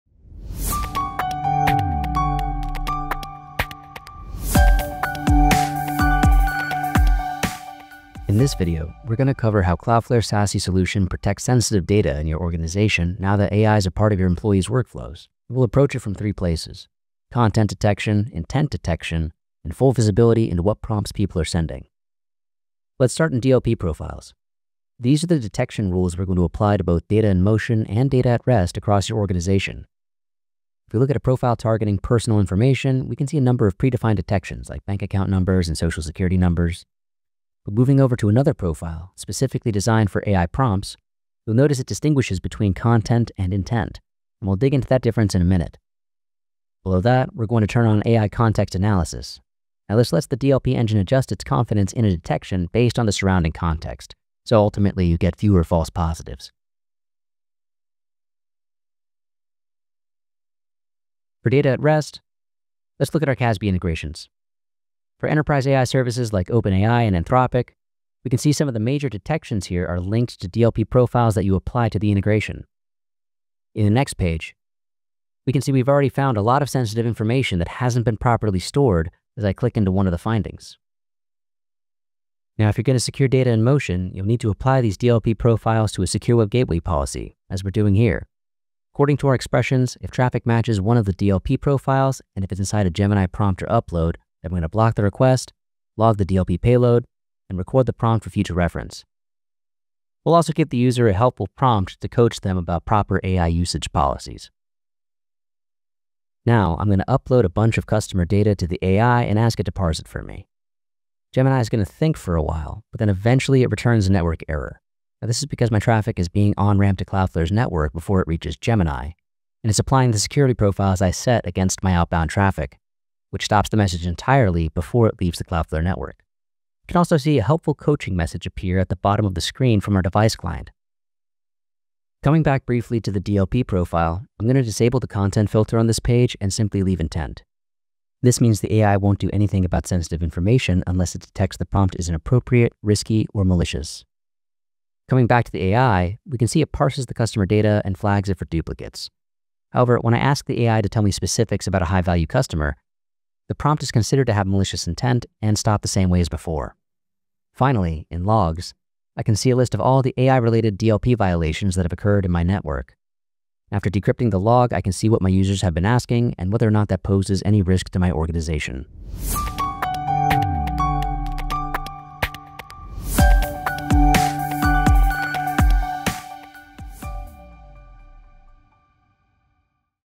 Demo: Prevent data exposure in AI
See how Cloudflare One helps protect sensitive data when users interact with generative AI apps. This demo highlights secure access service edge (SASE) capabilities including:
Data loss prevention (DLP) detections for sensitive content (e.g., PII, source code, financials)  0:22
Detections for data at rest in AI tools like ChatGPT 1:00
Guardrails for user prompts based on intent / topic to block jailbreak attempts, code abuse, PII requests, and other risky behavior 2:12